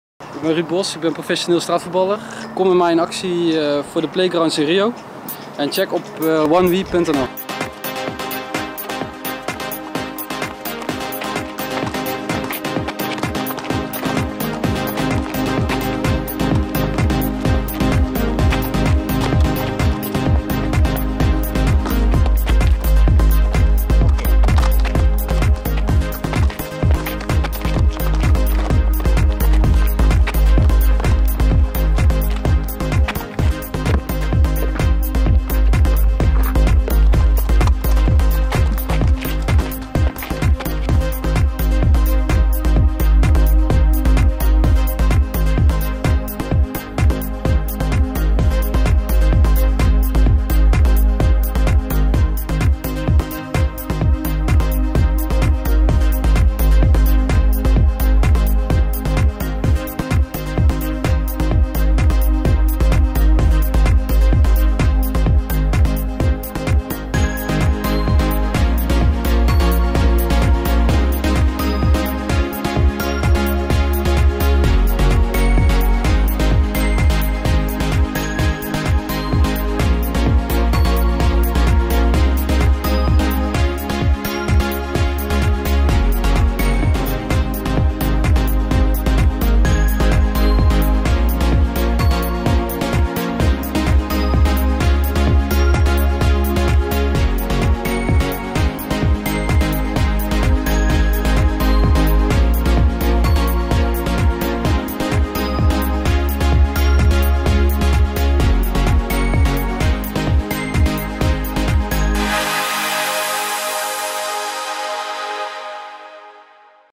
1We PANNA VOETBAL - Help de kids in Rio!
Om de problematiek van kinderen en jongeren in onder meer de favela's van Rio onder de aandacht te brengen, start 1We - Een Wereld Idee de 1We Panna voetbalwedstrijden. Dit jaar vinden er tien wedstrijden plaats, verspreid door het land. Topstraatvoetballer Ruud Bos treedt hierbij op als 1We ambassadeur samen met zijn team 2Fast.

Panna voetbal is een vorm van straatvoetbal op een klein veldje of pleintje, waarbij twee teams van één tot vijf spelers elkaar de bal moeten aftroeven met behendige voetbaltrucs. Niet het scoren van een doelpunt staat voorop, maar mooie acties, respect afdwingen en de tegenstander aftroeven. Panna is ontstaan uit de Surinaamse voetbalcultuur. De letterlijke vertaling van het woord is 'poortje', wat in het voetbal staat voor het door de benen spelen van de tegenstander. Wanneer je een panna scoort bij je tegenstander, betekent dit direct 'knock-out'. Het panna voetbal heeft de laatste jaren flink aan populariteit gewonnen. Hierdoor is het een perfect middel om de 1We Football Playgrounds onder de aandacht te brengen.

De 1We Panna voetbalwedstrijden vinden plaats op verrassende locaties, dwars door Nederland. De populariteit van deze vorm van voetbal, in combinatie met korte clinics door Ruud Bos en zijn team, trekt gegarandeerd een groot publiek. Tijdens de wedstrijden, zet 1We een promotieteam in. 1We - Een Wereld Idee wil met dit initiatief vooral kinderen en jongeren bewust maken van de situatie van leeftijdgenoten in achtergestelde gebieden. Én tegelijkertijd geld genereren voor de 1We Football Playgrounds.